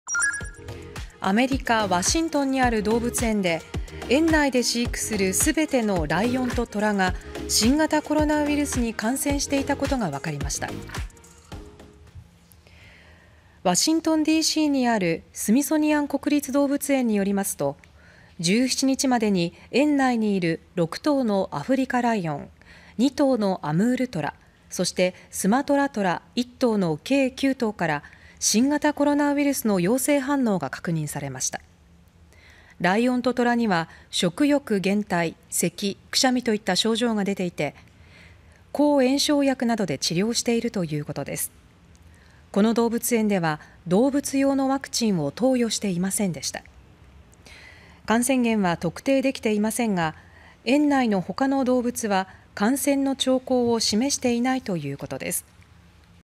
米の動物園で園内全てのライオン、トラがコロナ感染(2021年9月18日)
アメリカの動物園で園内のすべてのライオンとトラが新型コロナウイルスに感染していたことが分かりました。

　ワシントンDCにあるスミソニアン国立動物園によりますと、17日までに園内にいる6頭のアフリカライオン、2頭のアムールトラ、そしてスマトラトラの計9頭から新型コロナウイルスの陽性反応が確認されました。

　ライオンとトラには食欲減退、せき、くしゃみといった症状が出ていて、抗炎症薬などで治療を受けています。

　この動物園では動物用のワクチンを投与していませんでした。

　感染源は特定できていませんが、園内の他の動物は感染の兆候を示していないということです。